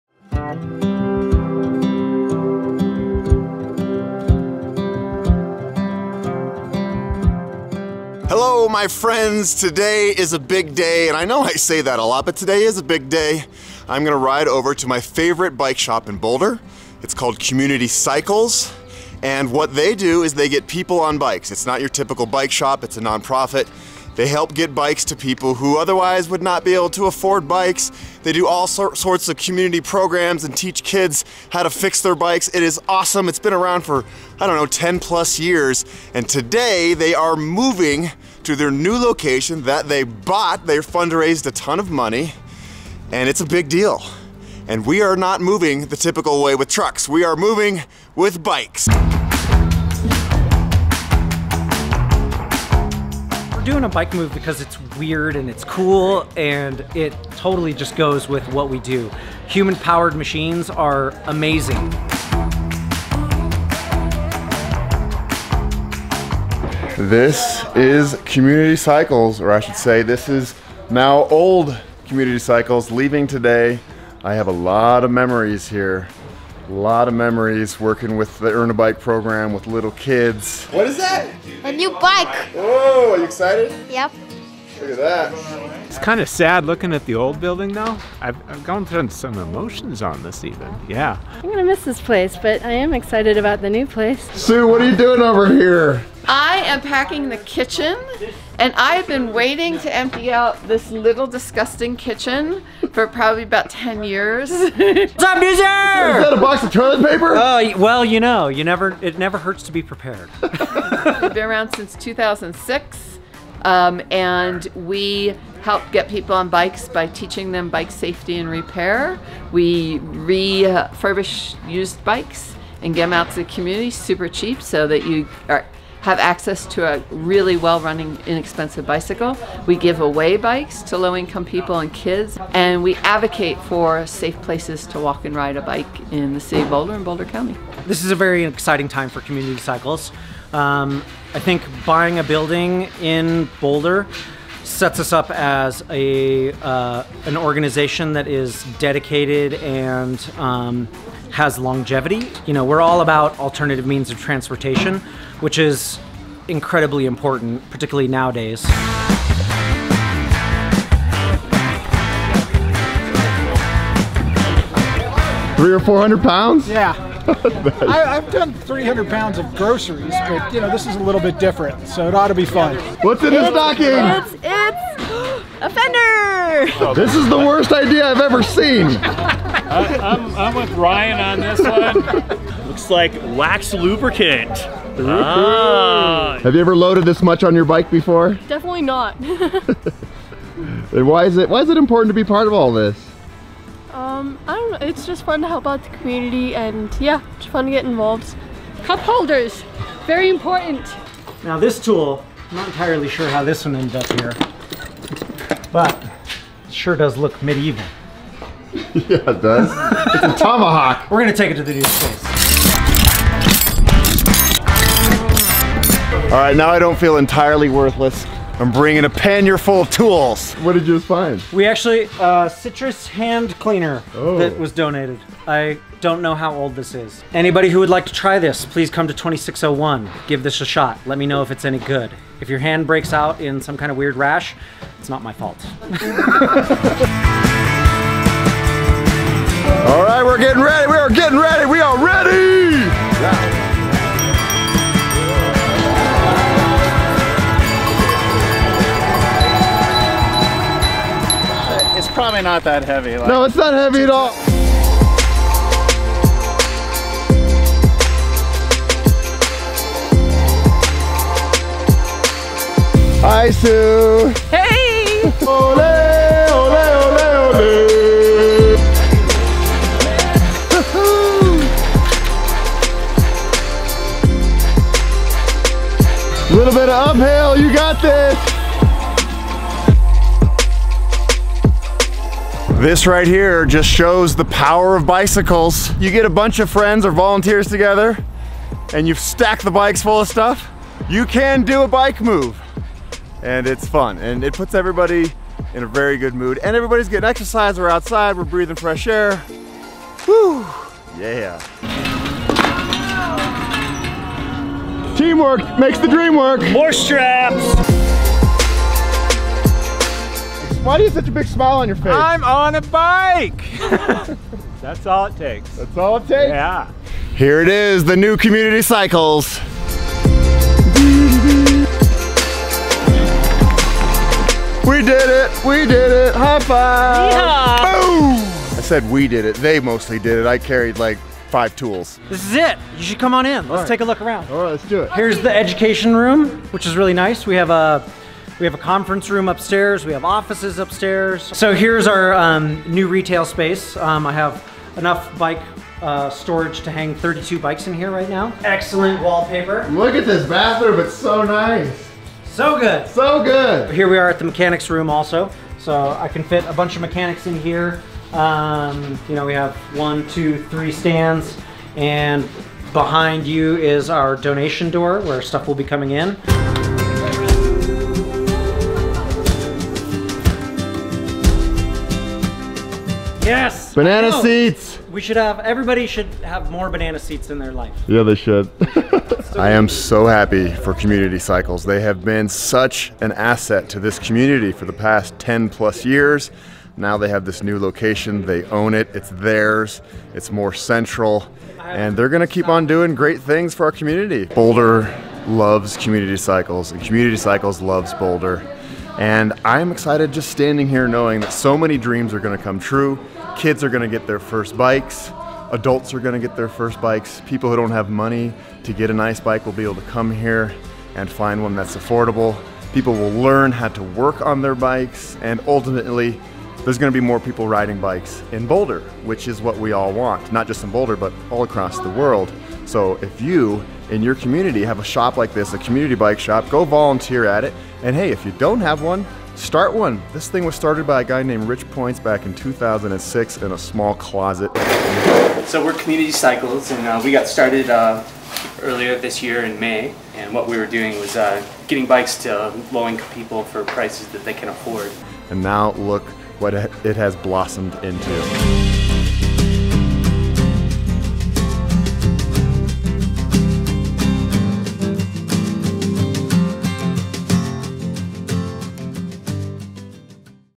We Used Cargo Bikes to Move An Entire Bike Shop-Community Cycles!
My favorite bike shop in the world, Community Cycles, has a new home! Instead of use cars and trucks to move all the stuff, we used an army of volunteers with cargo bikes and trailers. Community Cycles has been serving Boulder since 2006, promoting, encouraging and celebrating cycling. To learn more about them, go to:

Camera Gear:
Sony A6500
Sony 18-105 lens
Sony Action Cam FDR-X-3000

*I do get some compensation if you choose to sign up, yippee!